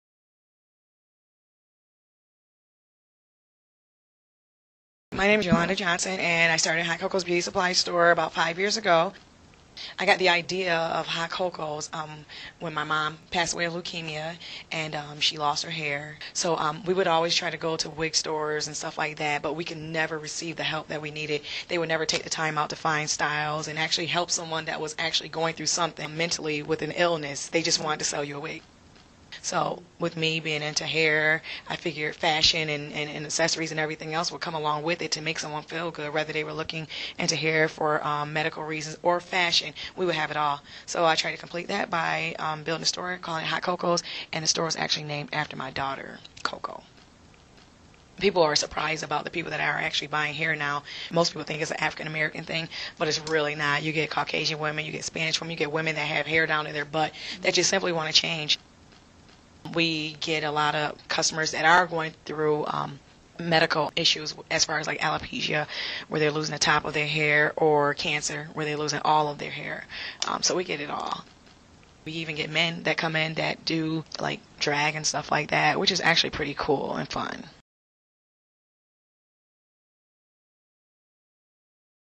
Cocoa's Beauty Shop
Hot Cocoa's Beauty Supply and Boutique has something that can not only help someone find a new look, but can also help someone cope with a debilitating illness. The beauty shop and accessories store has hundreds of wigs of all colors and styles, from a dark brunette bob to neon pink curls.

Produced by Jillian Anthony, S.I. Newhouse School of Public Communications for the Secs in the City project.